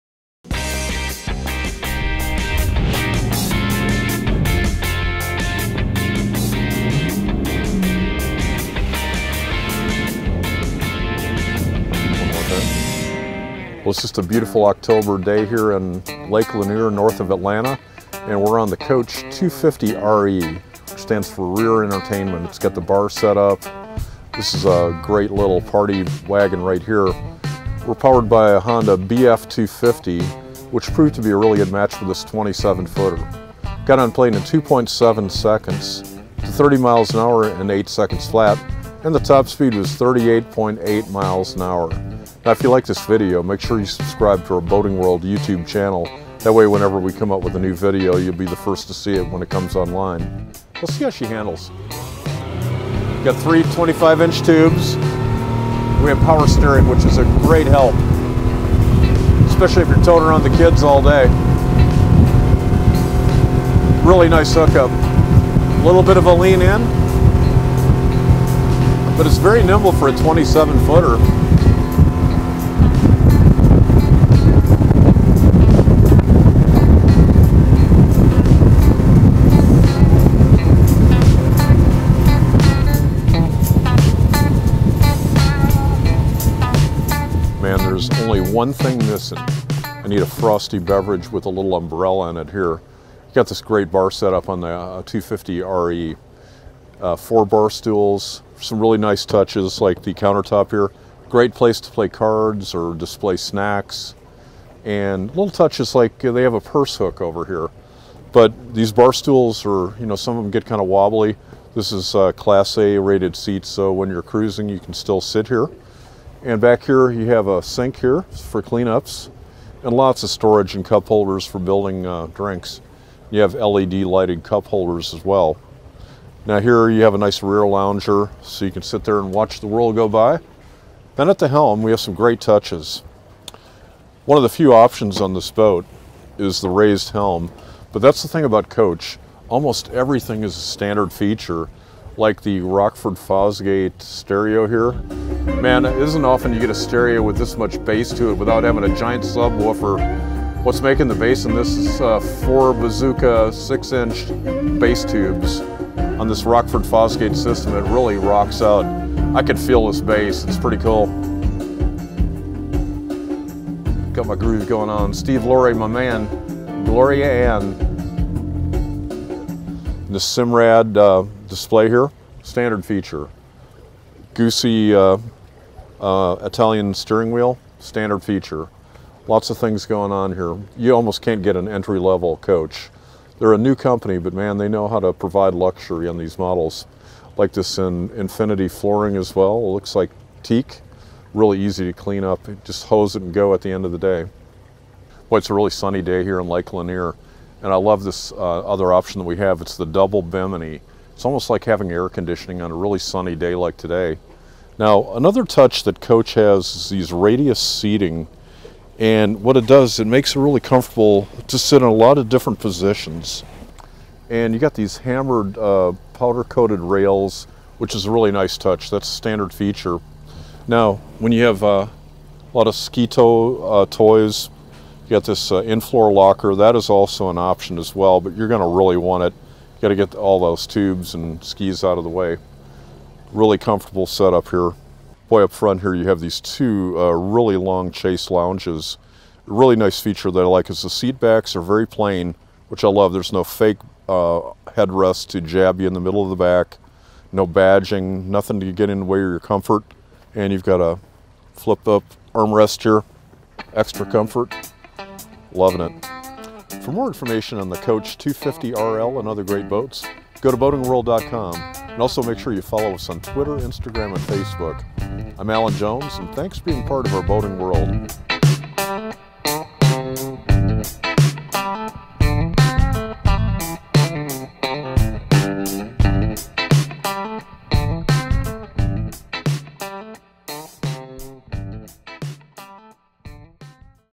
Coach 250 RE
With a single Honda BF250, the Coach 250 RE can reach almost 40 mph and accommodate a day of watersports or an evening cocktail cruise with a large group.

For more boat tests like this one, and many more, go